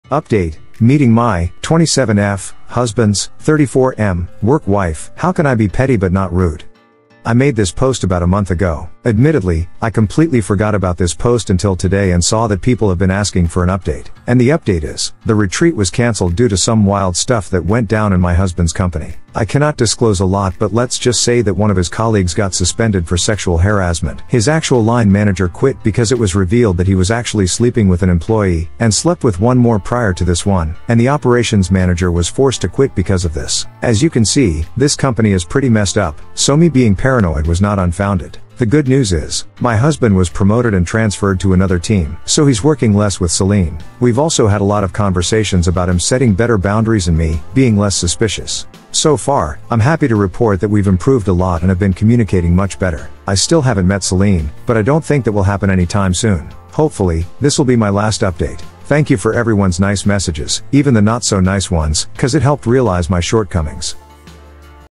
[Update] Meeting my (27F) husband's (34M) work wife, how can I be petty but not rude?
author: u/Interesting-War-9800